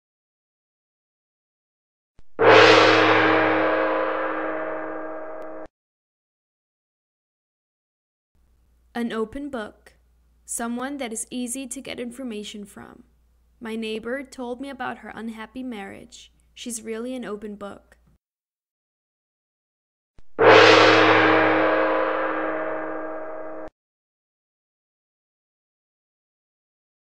An Open Book | Ask Linda! | Idioms
The idiom "an open book" refers to someone that is easy to get information from. A good example would be: My neighbor told me about her unhappy marriage. She really is an open book. Can you use this idiom in an example in the comments below?